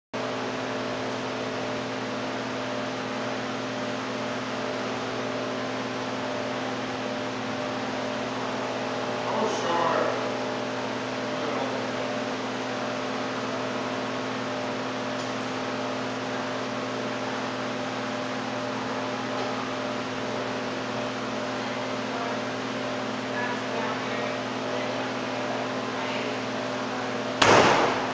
Flying Trapeze_Dino_playingonthebar
f/e uprise, straddle uprise, planche turn around, arch to miss the board, reverse suicide.